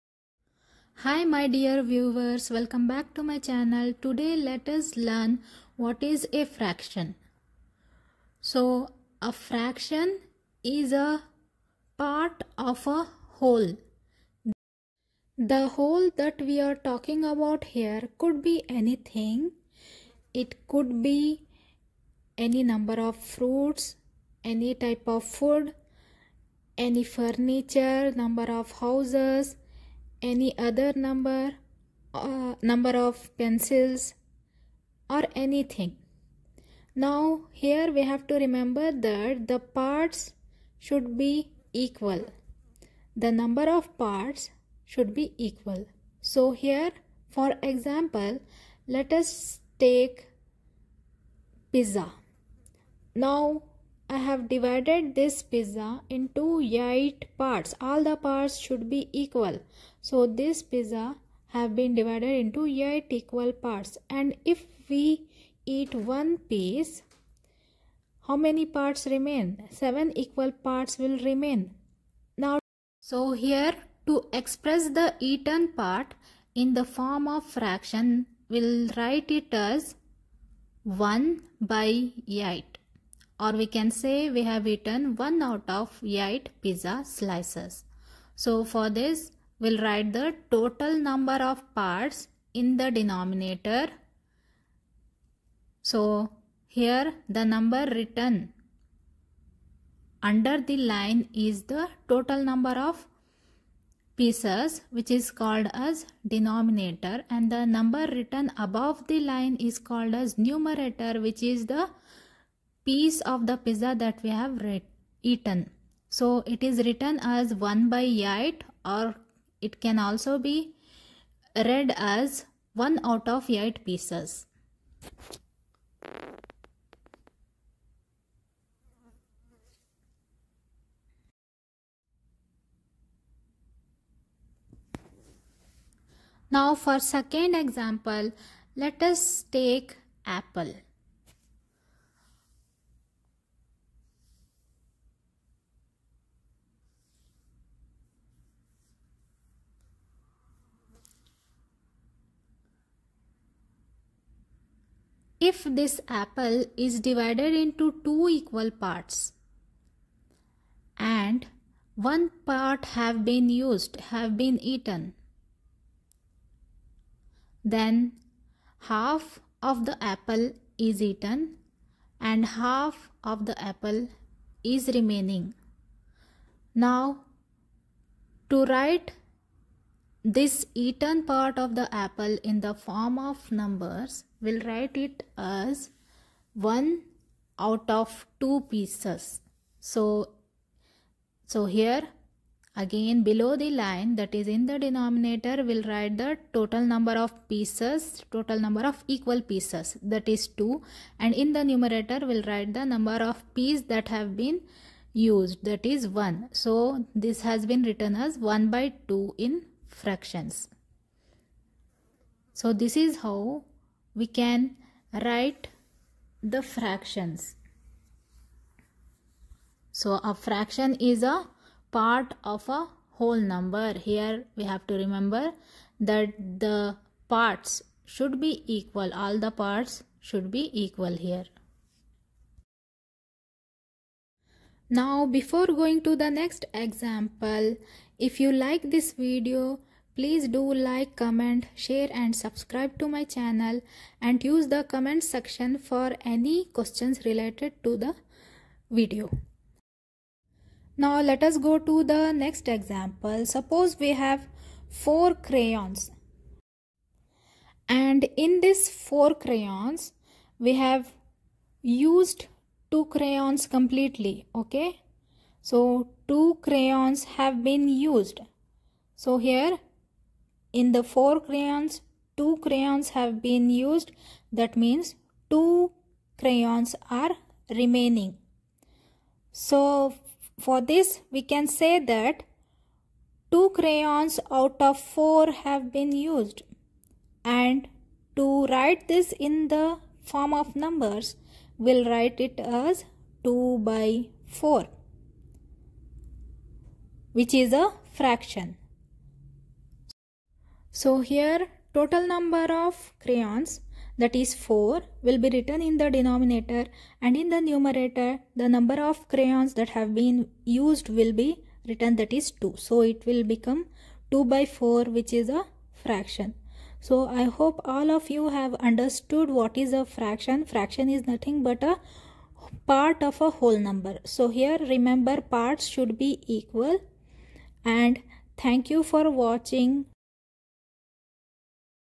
What is a Fraction? | Introduction to Fractions | Basic concept of Fractions in English
This video contains Introduction to Fractions| What is a Fraction?| Expressing Fractions | Basic concept of Fractions. Use the comments section for any questions related to the video.